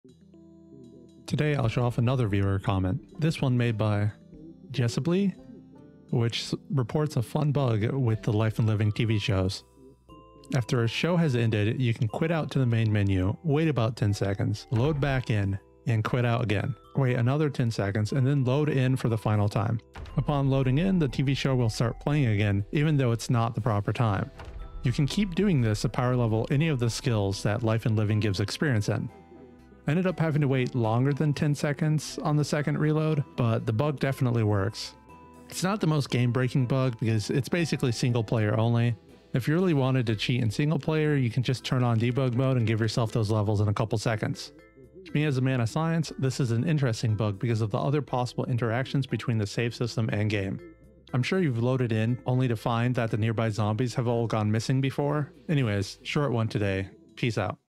Time Loop The TV Bug
Quickly showing off a bug a viewer commented. After watching a life and living episode you can quit to the main menu, load back and quit out again. Wait longer than 10 seconds and the load back in. Upon loading the TV will be playing that episode of life and living again. Not a huge bug, but entertaining enough. The real interesting thing to me is the potential for other save system shenanigans.